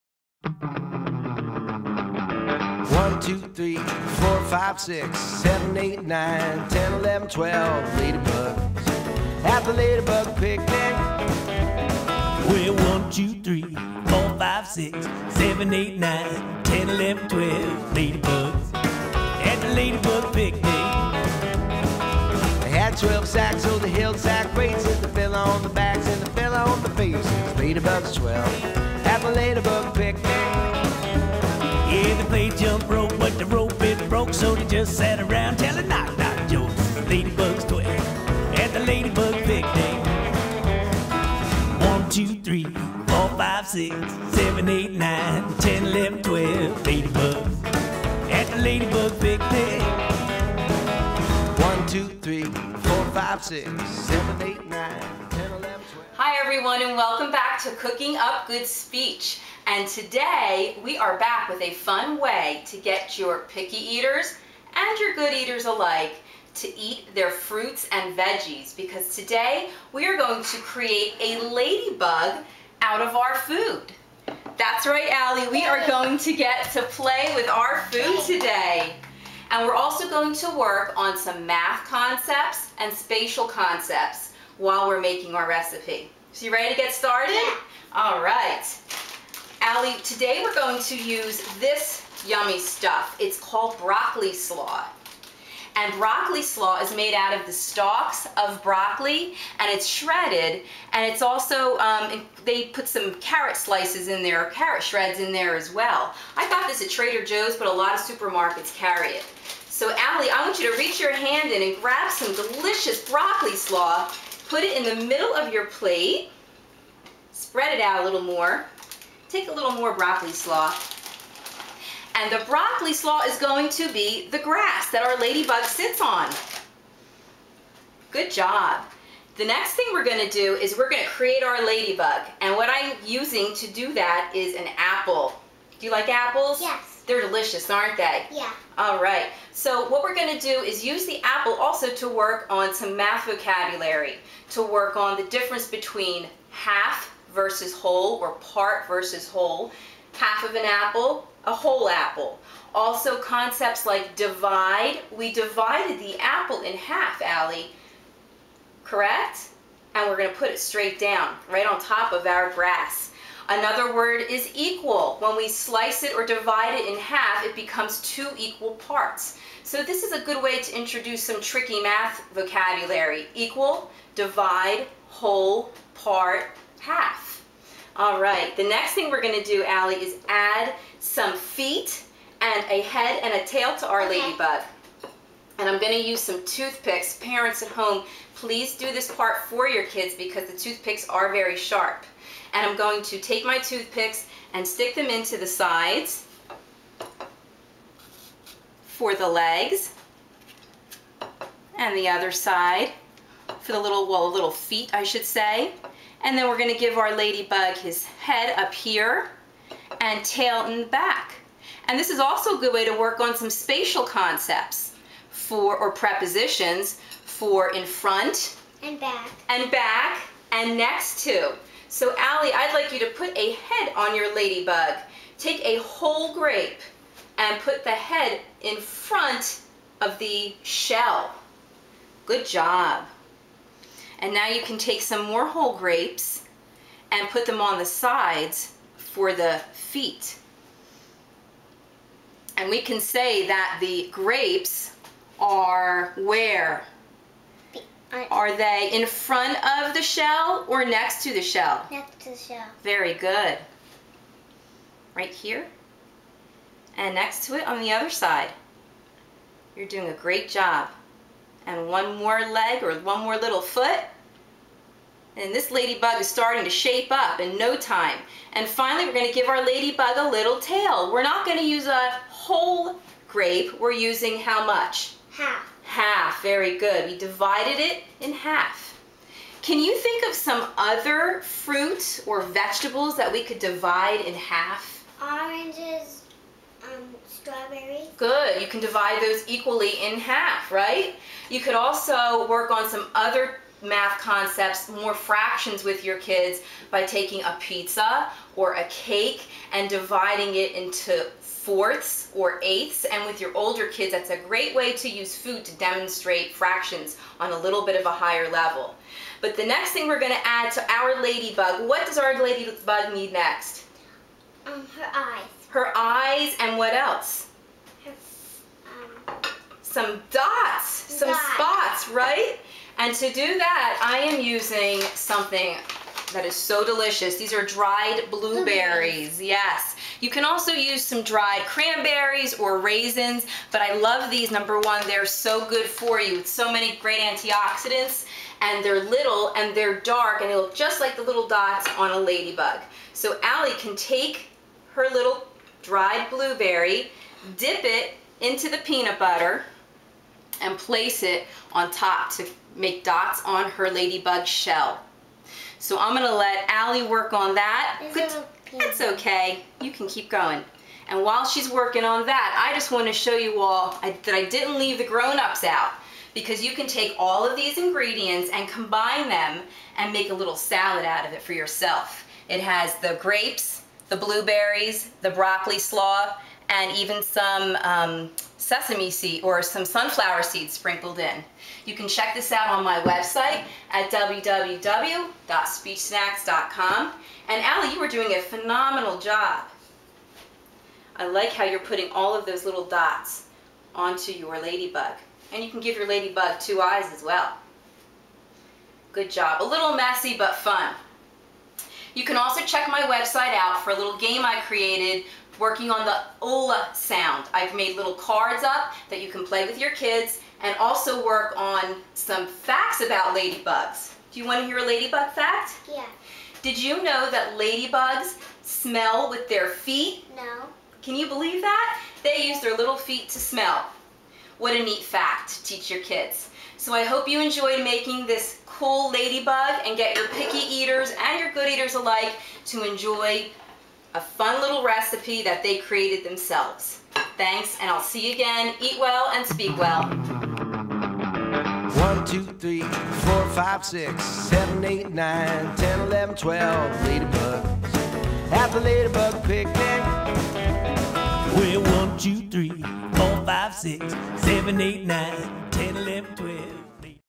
Speech Snacks for Ladybug Salad
Make a healthy fruit and veggie treat that looks like a ladybug while addressing speech and language skills.    Perfect for the classroom, life skills and home school--for all age levels!